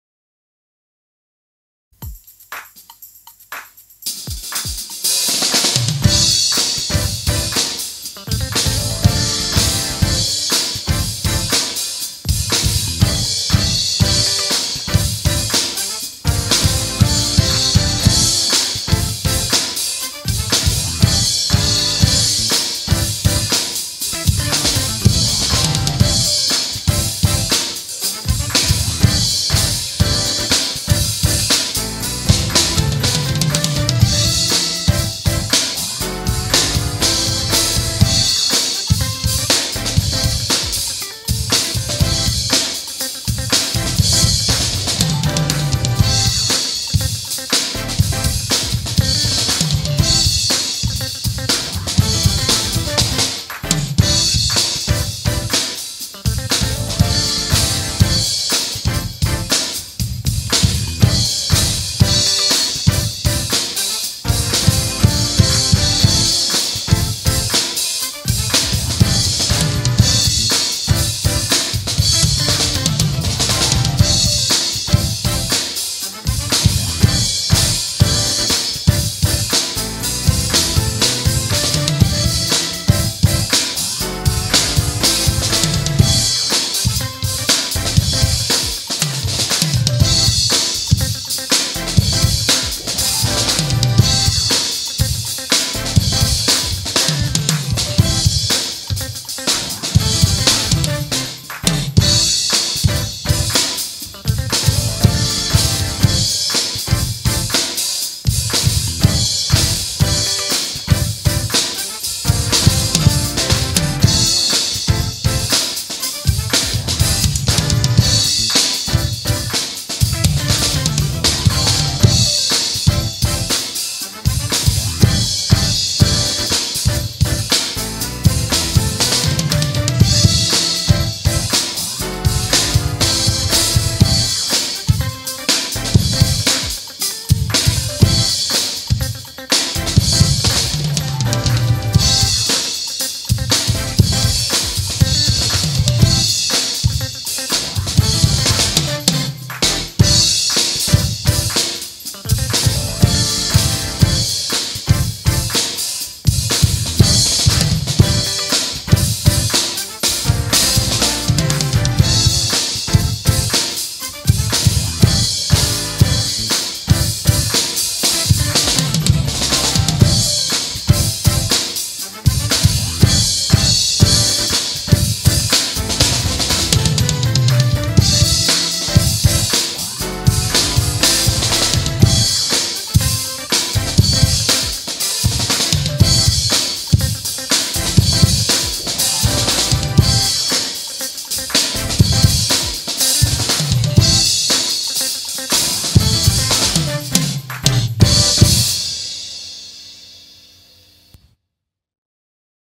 Drum Groove
Exploring my next level of Drumming. First Time to play on a Gospel Groove track.